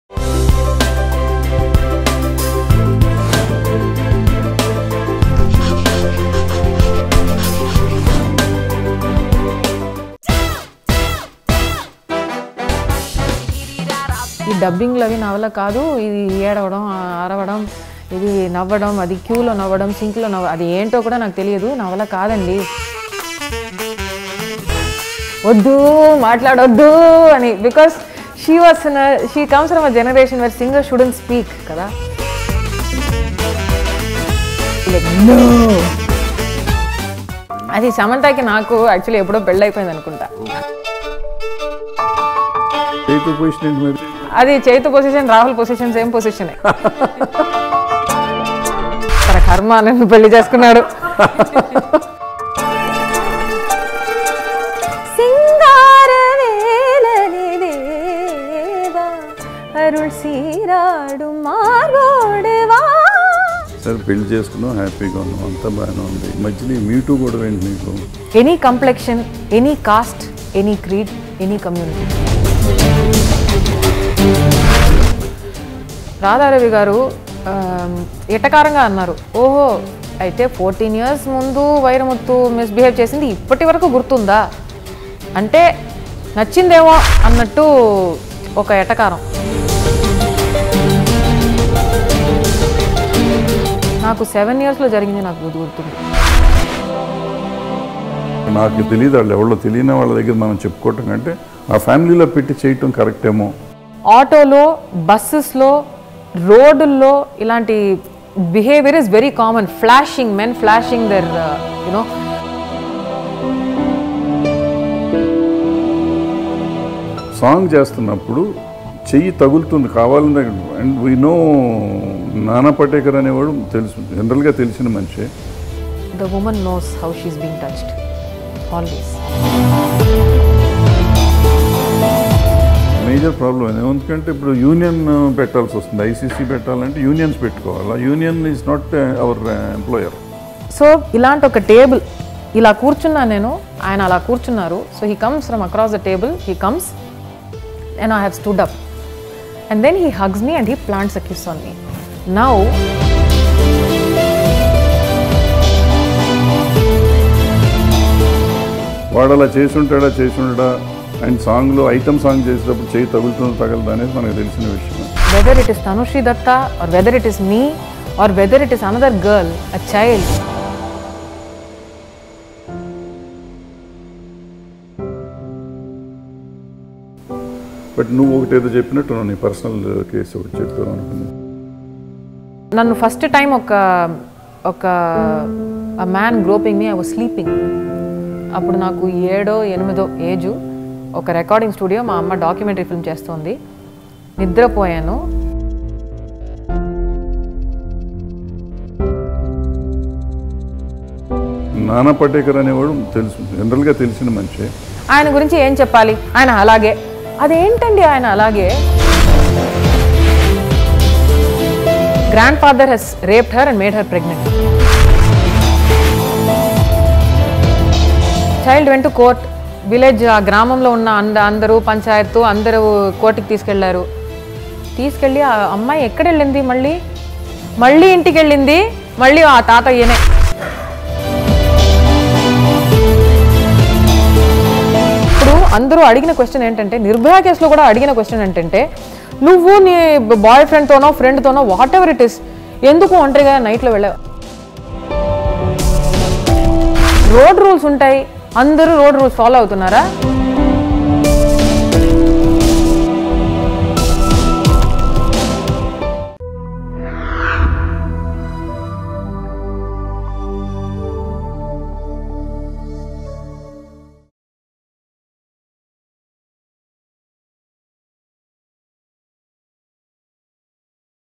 Chinmayi Interview | Tammareddy Bharadwaj Interview With Tollywood Singer Chinmayi
Chinmayi Interview with Telugu Veteran Director Tammareddy Bharadwaj. Tollywood Singer ChinmayiSripaada reveals a few interesting facts about Samantha, Naga Chaitanya & Rahul Ravindran. She also shares her personal life, tragedy moments which she faced in her life and opens up about the dark side of the South Indian Film Industry.

👉 Do share your views in the comments section below.

▶ Tammareddy Bharadwaj Reacts on Naga Babu Over Comments on Balakrishna | Nagababu Vs Balayya